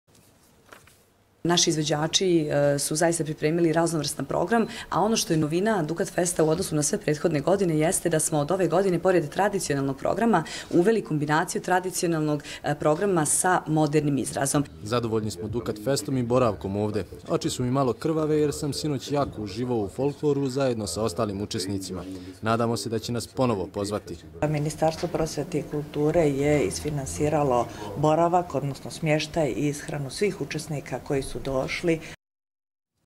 Prijem u Ministarstvu za učesnike Dukatfesta
Predstavnici Ministarstva prosvjete i kulture ugostili su učesnike ovogodišnjeg Devetog internacionalnog festivala kulture Dukatfest.